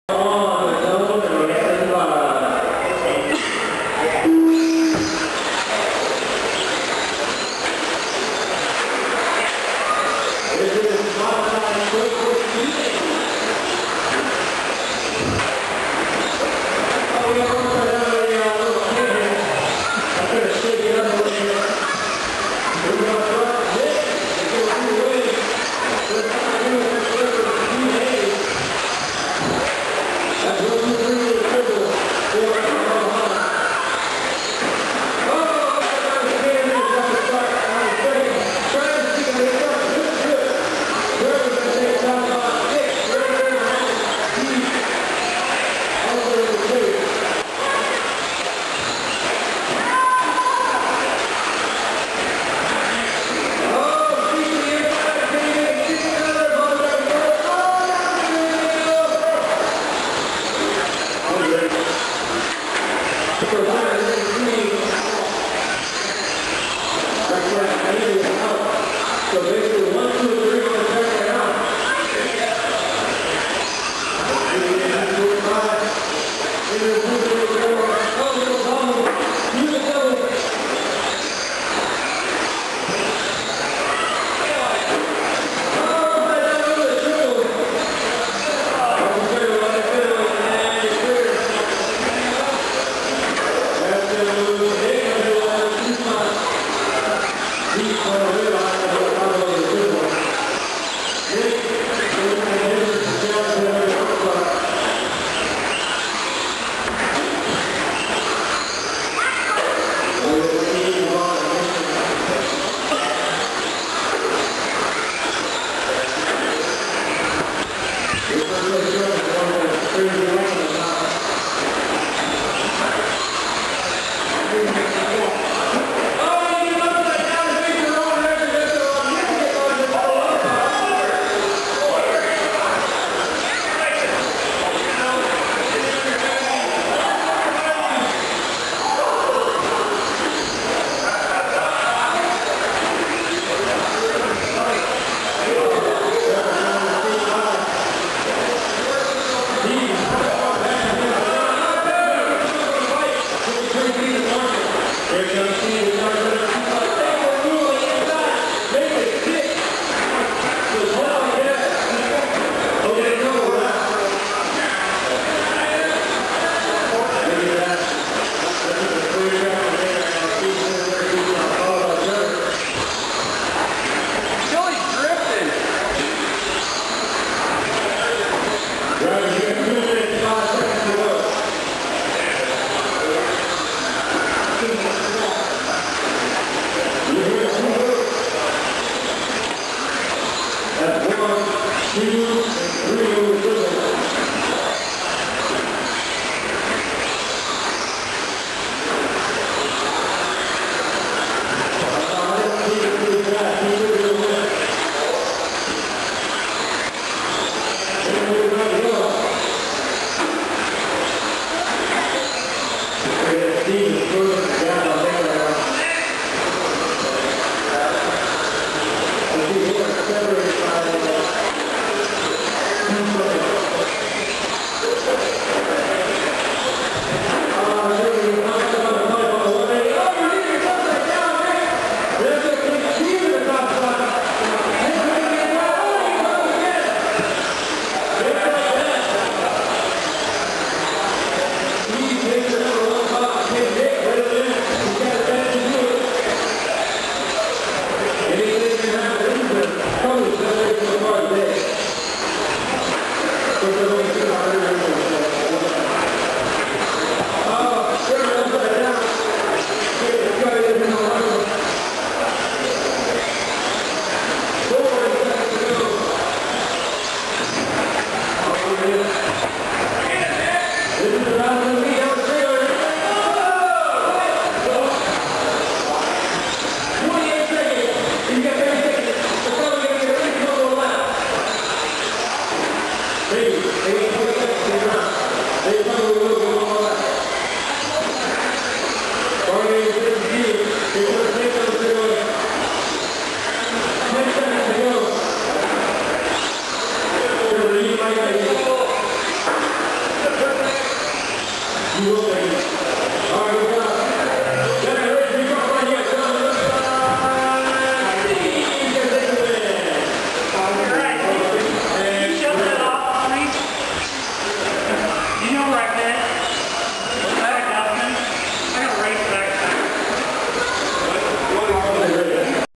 Southridge Shootout - 2wd Mod Short Course B Main - Des Moines,Iowa 4-2-2011
B Main racing action at the Southridge Shootout. Offroad R/C racing indoors at The Electric Pit in Southridge Mall Des Moines, Iowa on April 2, 2011 Results